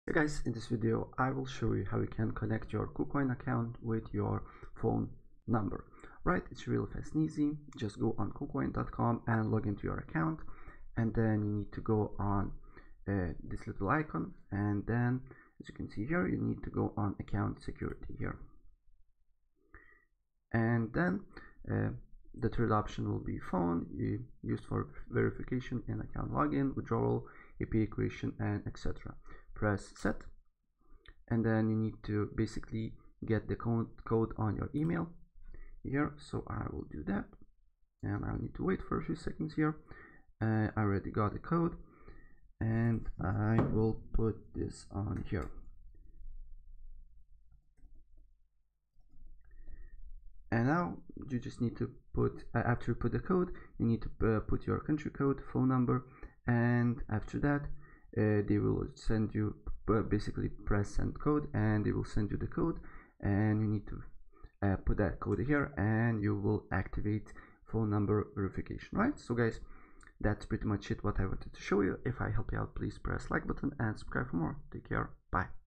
How To Connect Kucoin To Your Phone Number
In this video I will show you how to connect kucoin to your phone number.  It's really easy and it will take you less than a minute to do so!